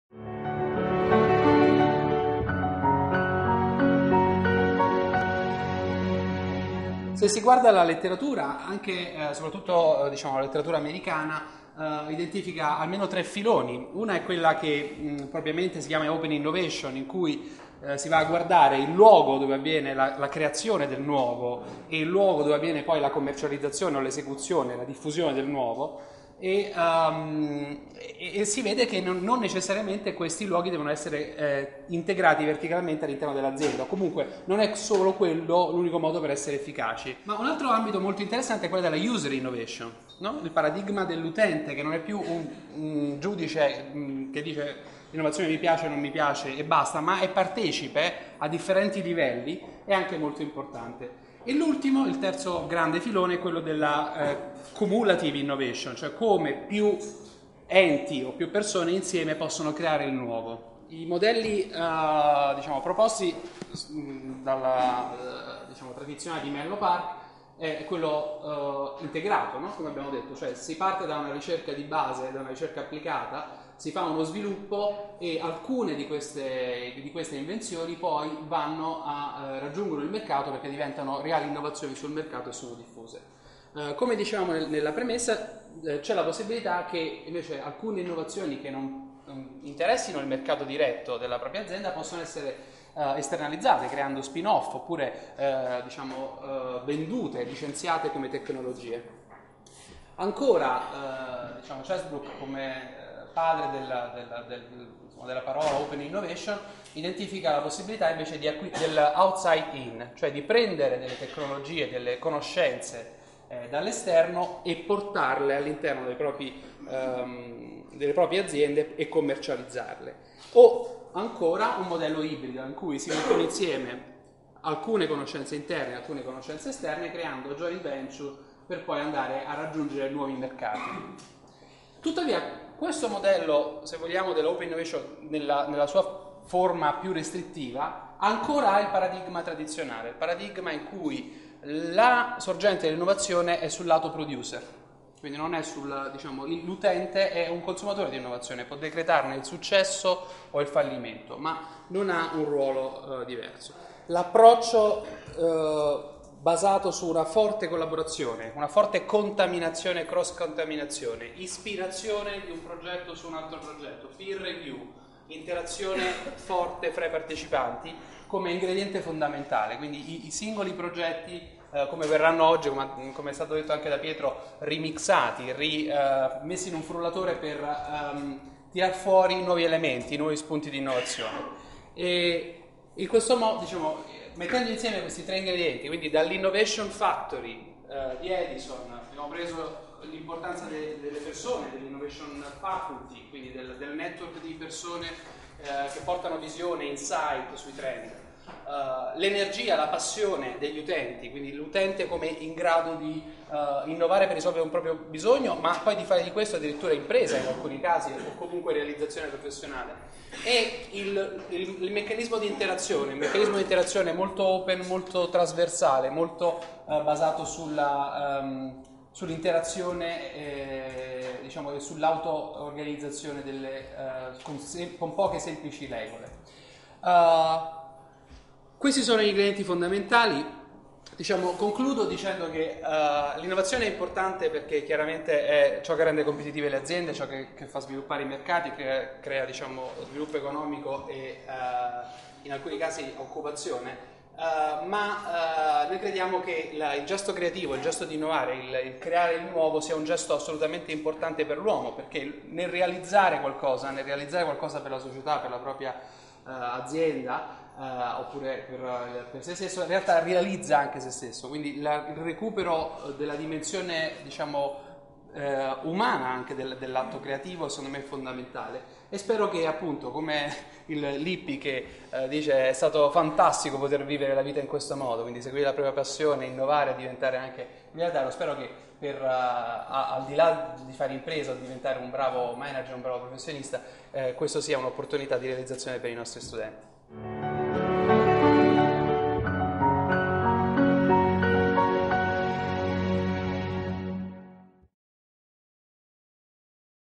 Innovation Day ELIS: intervento di Giuseppe Cinque
Il 3 febbraio 2011, 30 amministratori delegati e oltre 30 esperti d'innovazione delle principali aziende italiane e del Consorzio ELIS si sono incontrati nell'aula magna dell'ELIS per ragionare e lavorare su modelli d'innovazione nel Mobile Ecosystem. L'iniziativa lanciata nel semestre di presidenza Wind del consorzio ELIS ha presentato l'iniziativa Innovation Lab ELIS per il Mobile Ecosystem.